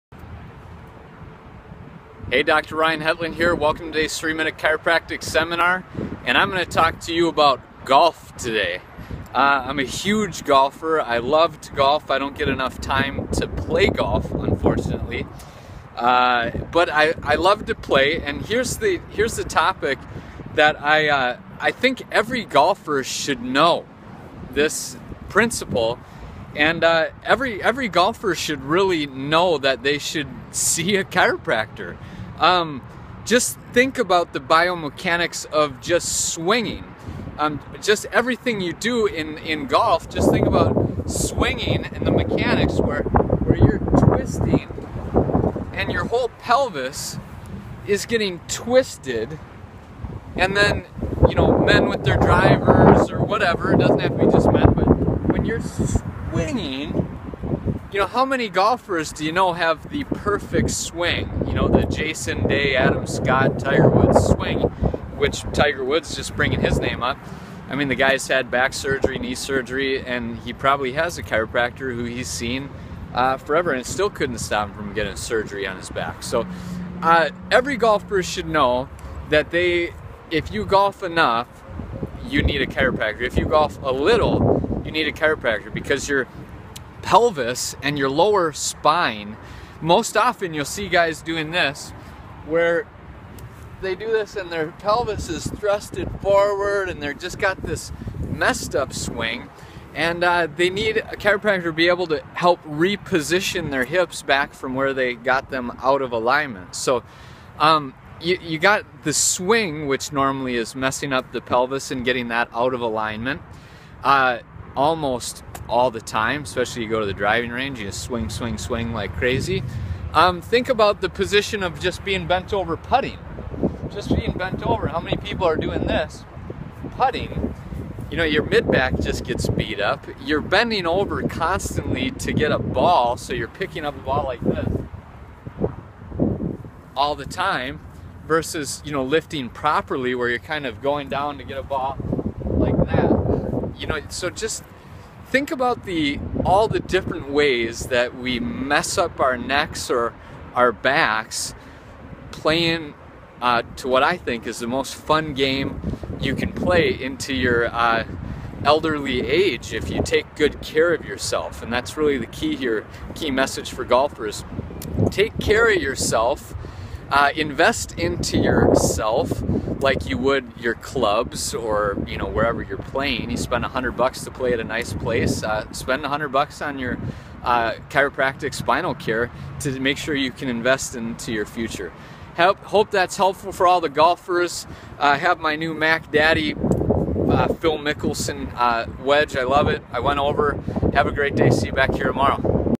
Why Every Golfer Should See a Chiropractor
The 3 Minute Chiropractic Seminar Video by Dr Ryan Hetland, Your Twin Cities Gonstead Chiropractor.

Visit one of my clinic locations in Burnsville or Inver Grove Heights, MN